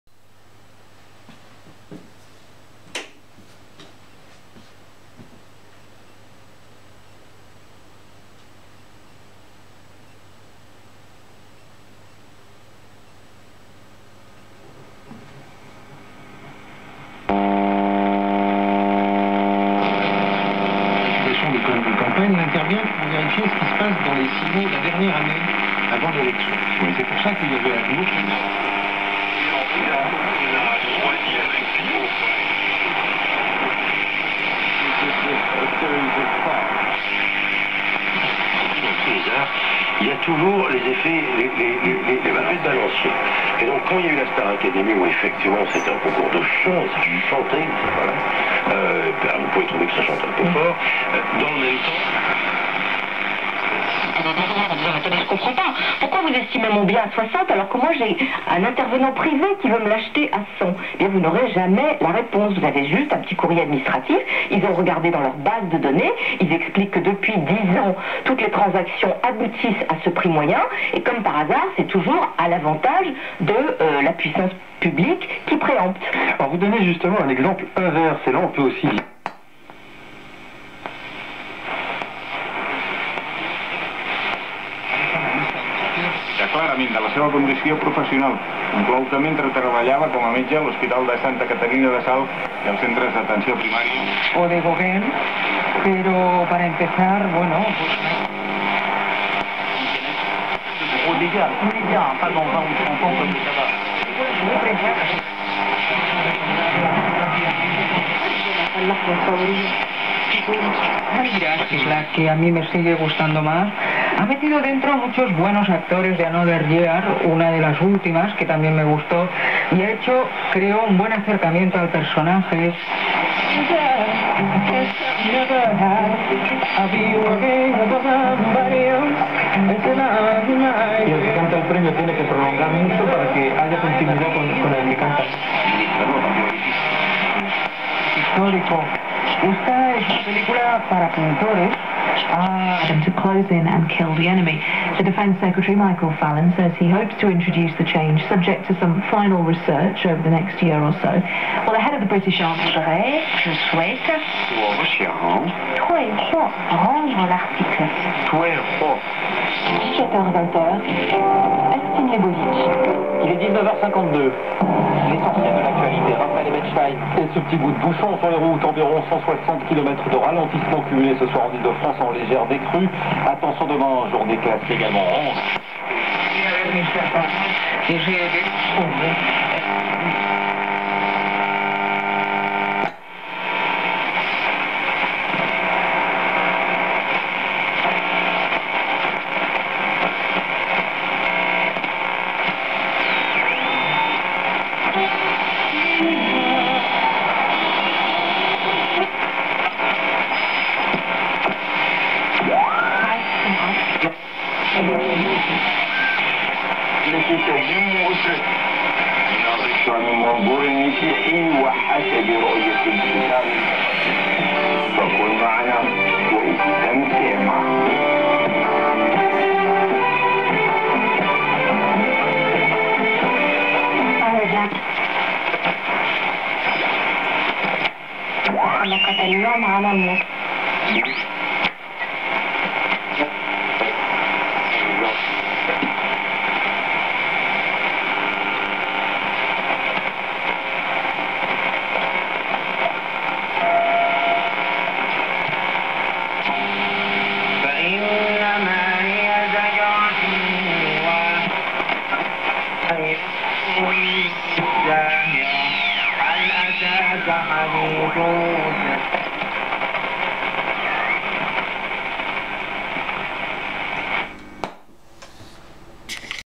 Restauration d'un ancien poste de radio TSF, LEMOUZY MYA
Jeu de lampes : 6A7, 6D6, 75, 42, 80
Cette radio possède un indicateur visuel d'accord, c'est l'ancêtre de l’œil magique.